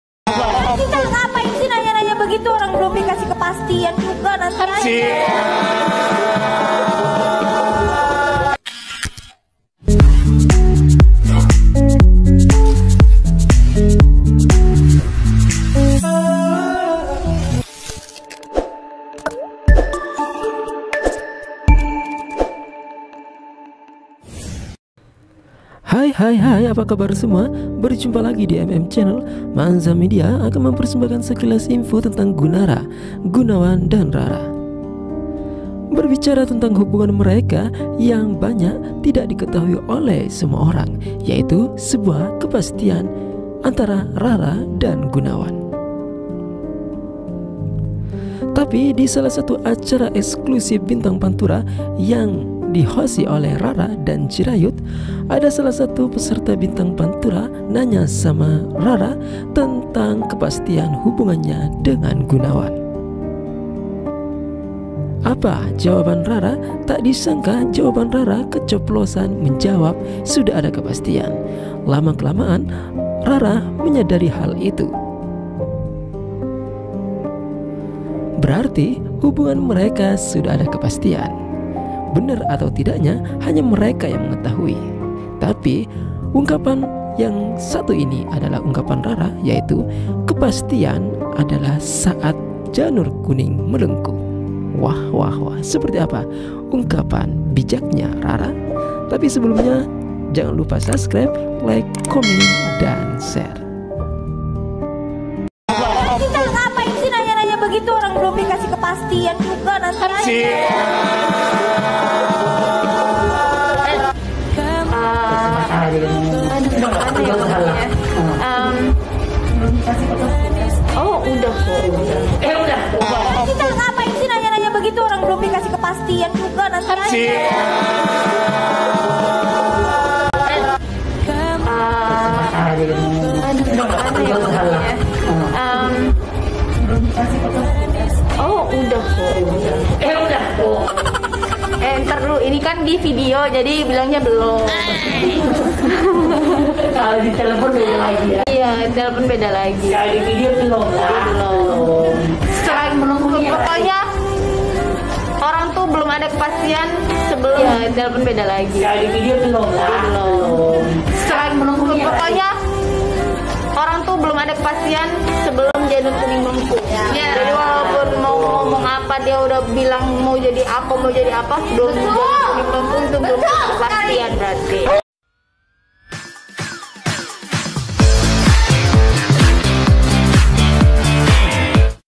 GUNARA..!! Buat Rara & Gunawan,, sebuah kepastian dalam Hubungan adalah Pernikahan
rara lida dan gunawan lida
kepastian adalah pernikahan itulah kata rara ..
cek vidionya ..